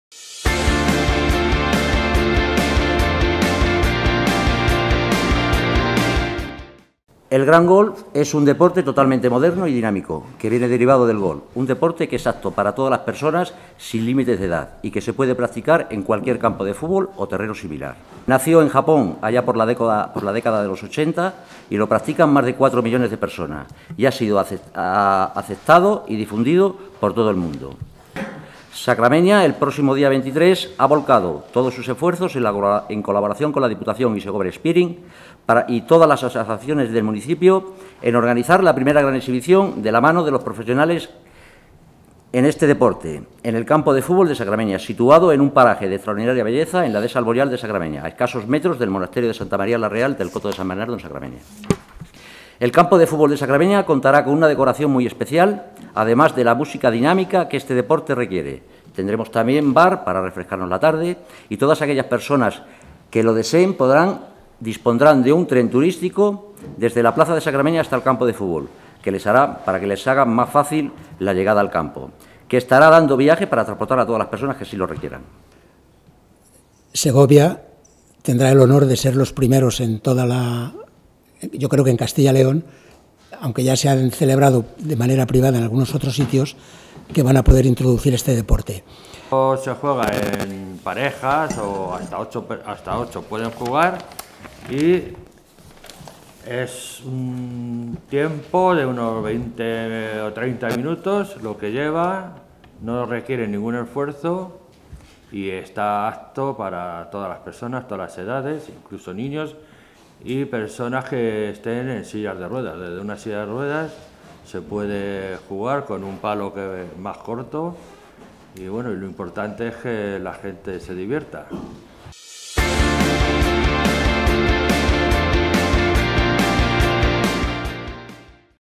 Sacramenia acoge el próximo sábado una exhibición del deporte `ground golf´
El campo de fútbol de El Prado de la localidad acogerá desde las cinco y media de la tarde a monitores especializados